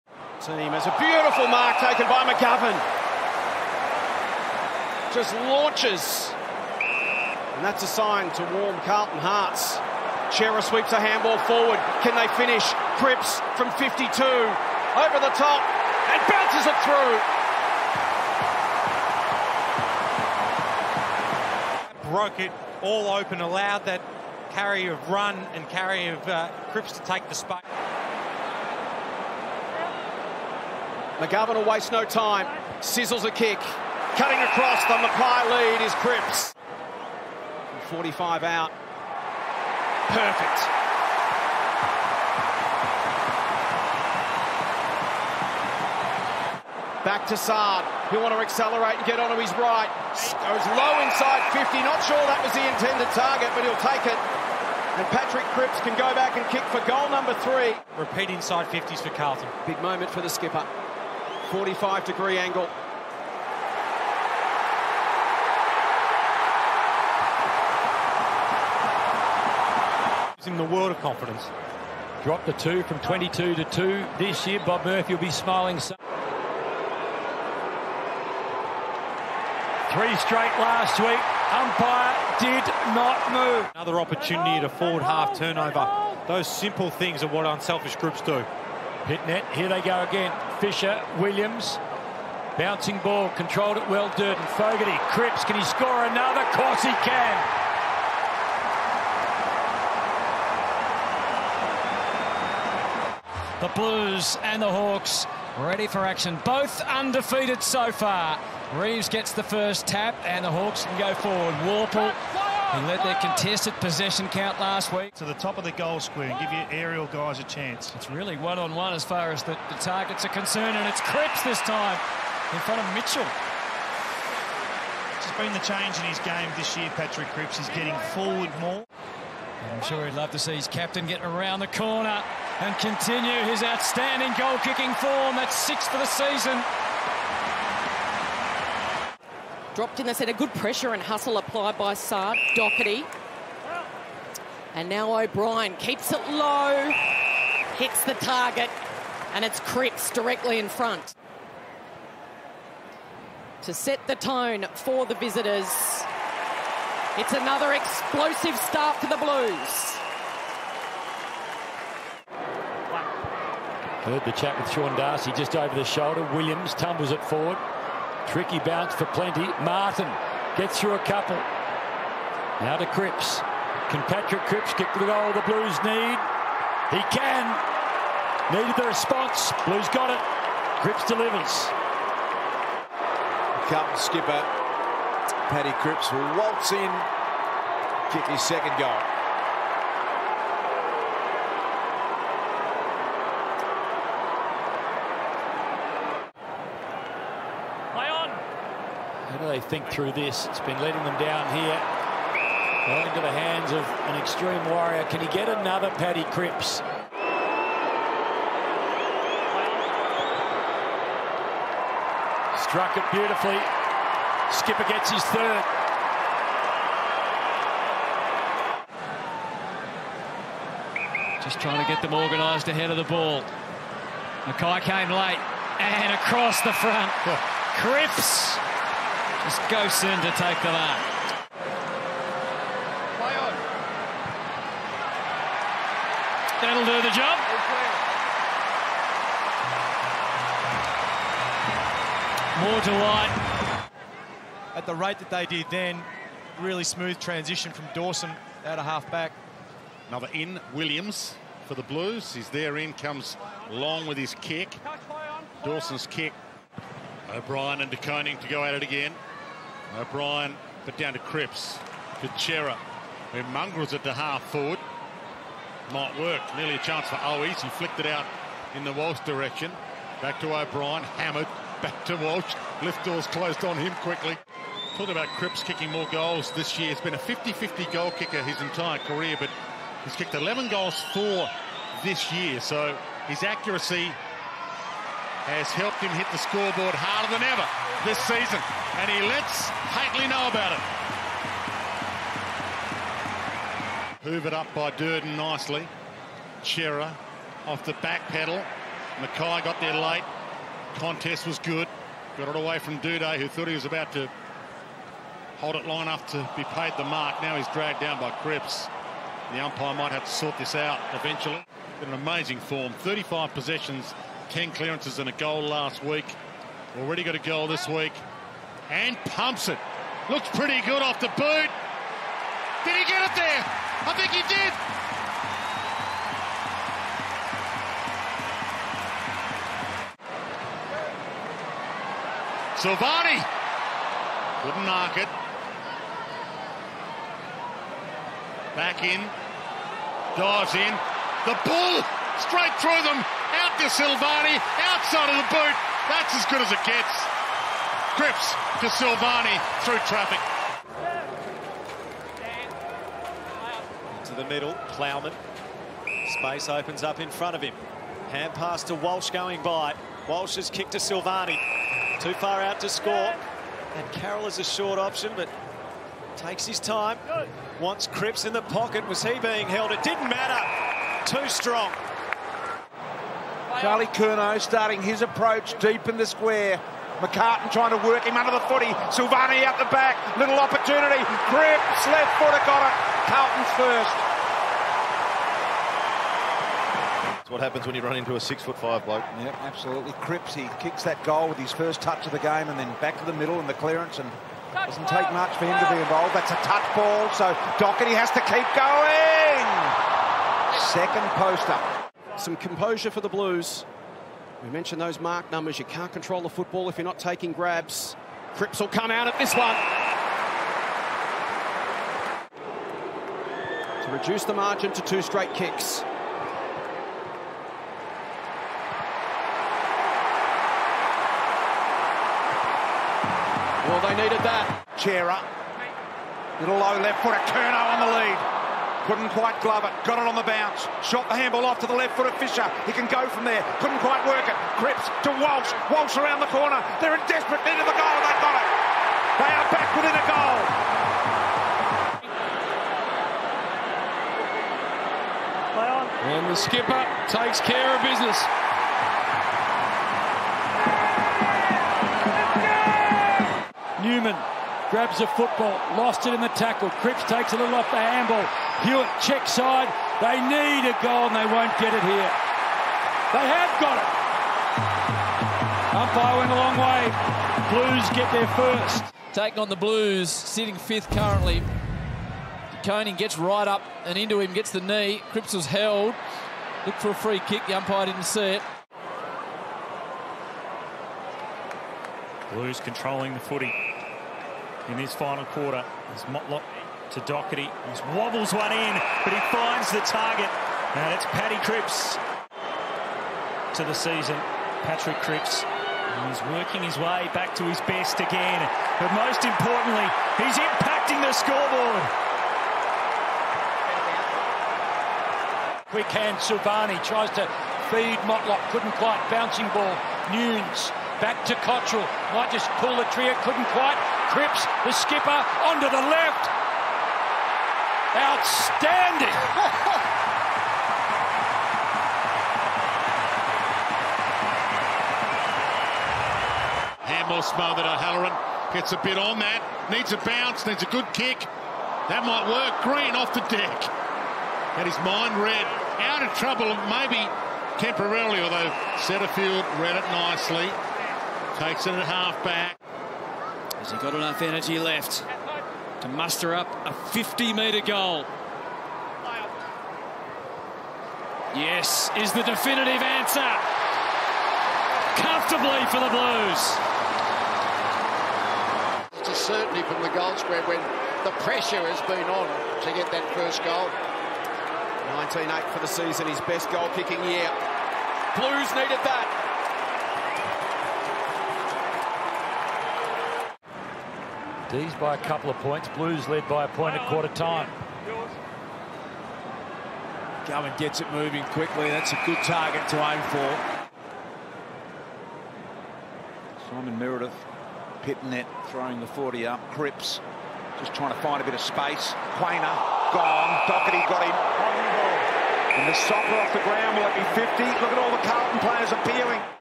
What an incredible year from Patrick Cripps! | Every club's best and fairest | AFL
A Brownlow and a club best and fairest. Patrick Cripps was simply incredible in 2022.